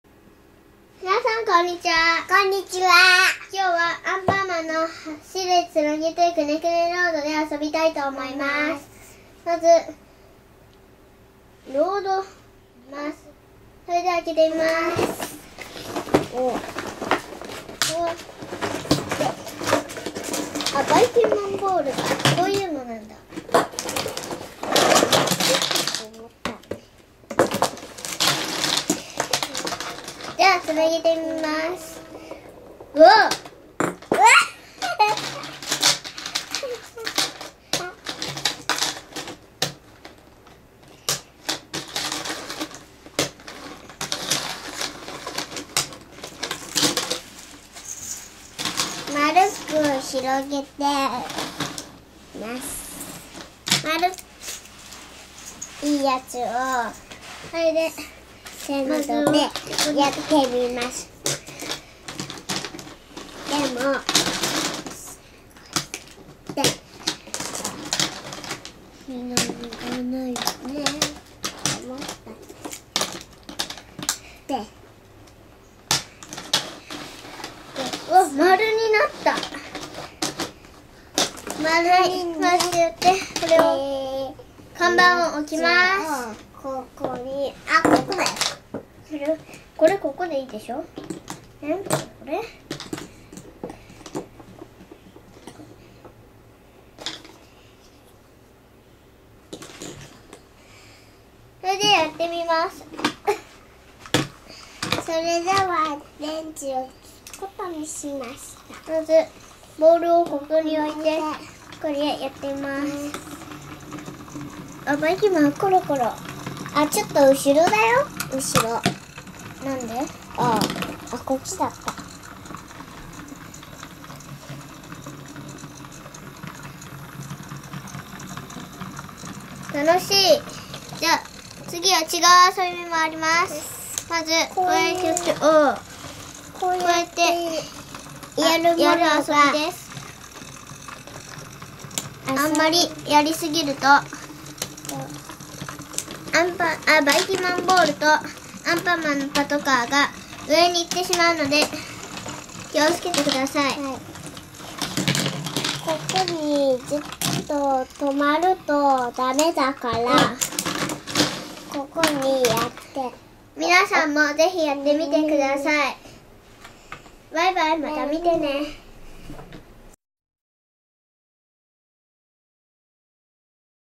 アンパンマン はしれパトカー！つなげてくねくねロード★anpanman Run, and a police car connects it; and is a way windingly
アンパンマンのはしれパトカー！つなげてくねくねロードで遊びました！
anpanman　Run, and a police car connects it; and is a way windingly